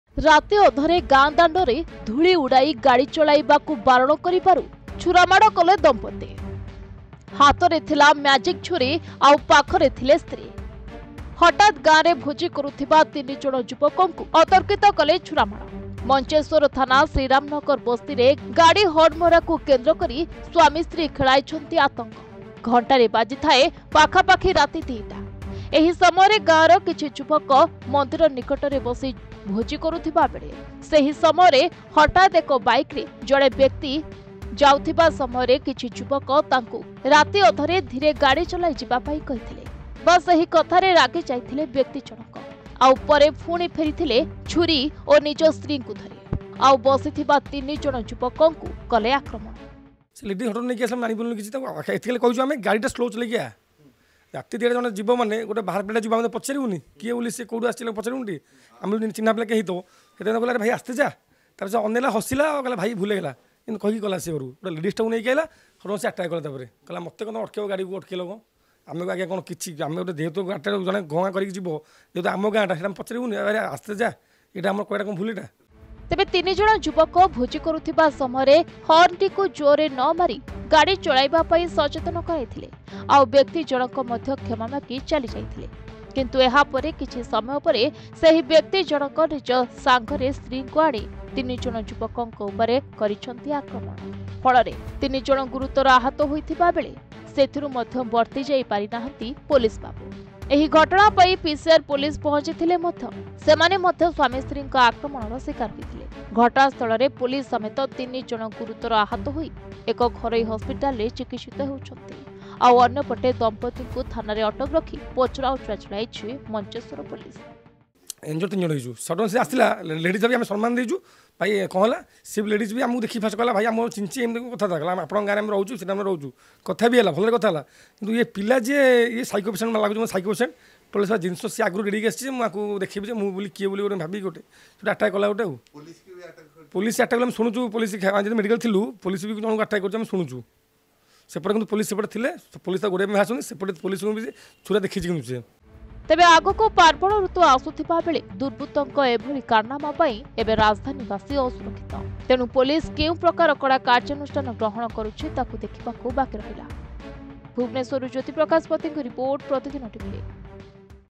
ଗାଡ଼ି ଟିକେ ଧୀରେ ଚଲାନ୍ତୁ କହିବାରୁ ,ପୋଲିସଙ୍କୁ ଛୁରା ମାଡ଼ କଲେ ସ୍ୱାମୀ ସ୍ତ୍ରୀ Mancheswar Police Attack Case
ଗାଡ଼ି ଟିକେ ଧୀରେ ଚଲାନ୍ତୁ କହିବାରୁ , ରାଗରେ ପୋଲିସଙ୍କୁ ଛୁରା ମାଡ଼ କଲେ ସ୍ୱାମୀ ସ୍ତ୍ରୀ l Mancheswar Police Attack Case